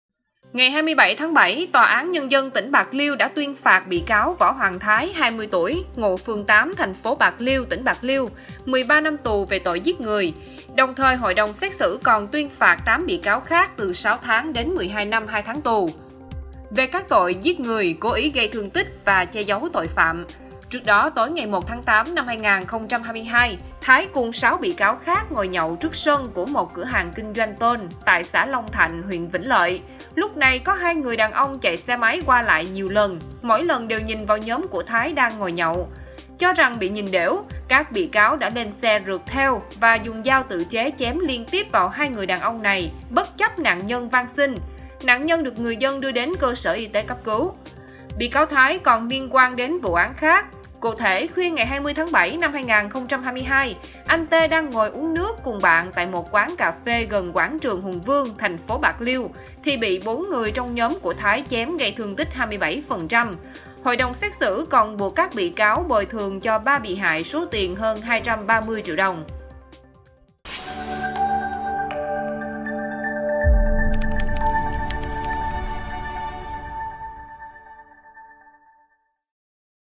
Cho rằng bị nhìn đểu, nhóm thanh niên dùng dao tự chế đuổi c.h.é.m người đi đường
Ngày 27-7, TAND tỉnh Bạc Liêu đã tuyên phạt bị cáo Võ Hoàng Thái (20 tuổi; ngụ phường 8, TP Bạc Liêu, tỉnh Bạc Liêu) 13 năm tù về tội “Giết người”.